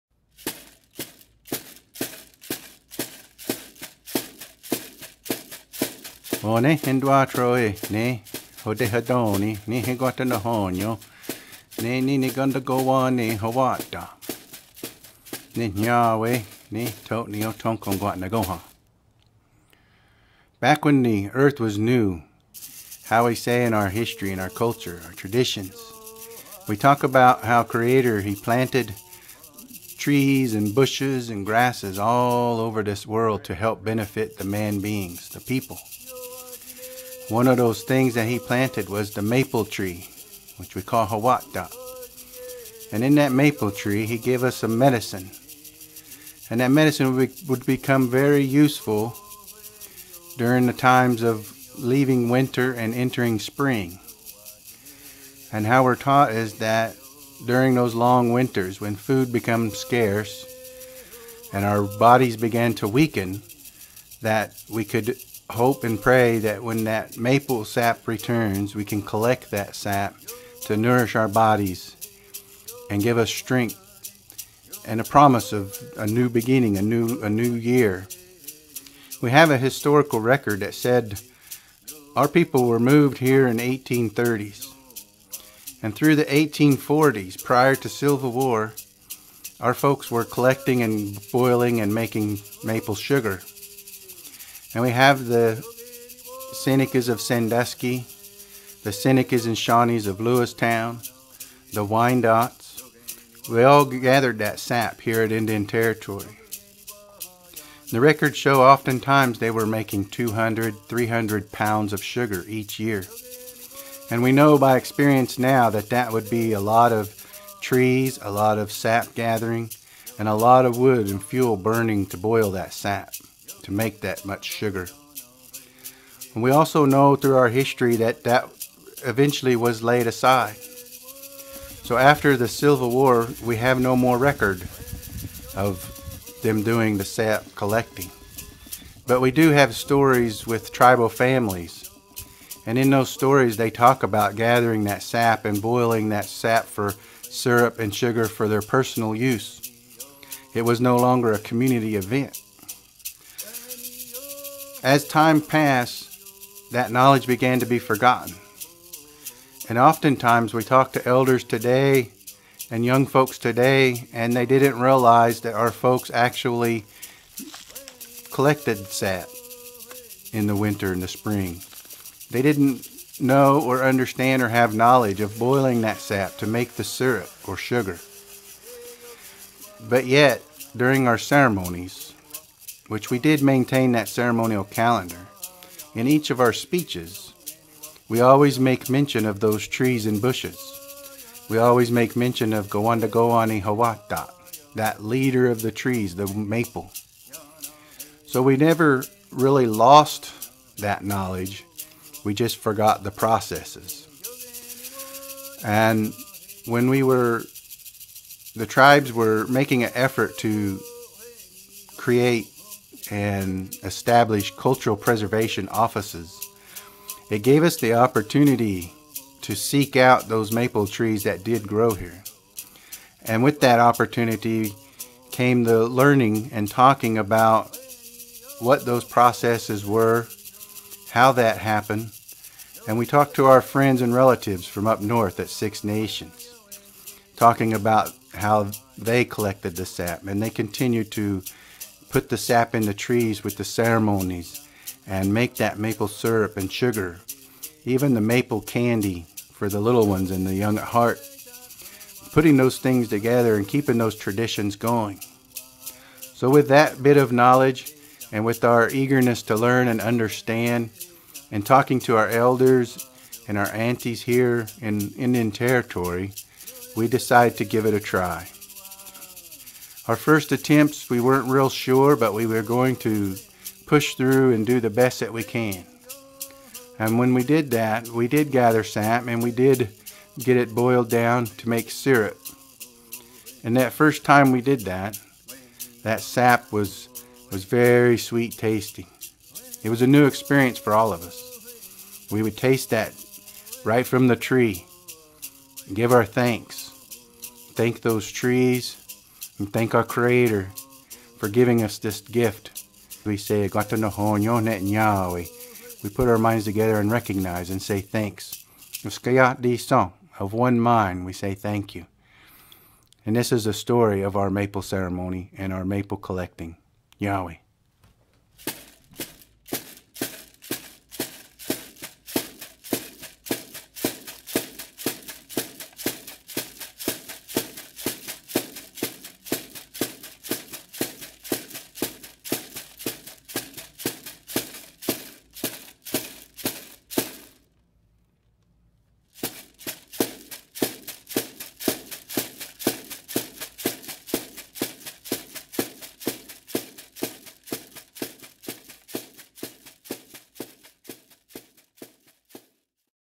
Sap Suckers
Native American tribes in Ottawa County, Oklahoma, continue the tradition of maple sugar making. Paul Barton (Seneca-Cayuga) shares a brief history of how they are rekindling a sweet part of their culture. Recorded during their twelfth year of collecting sap. From the video series, Lives We Weave, a Cultural Celebration, produced with a grant from Administration for Native Americans and Eastern Shawnee Tribe of Oklahoma in 2022.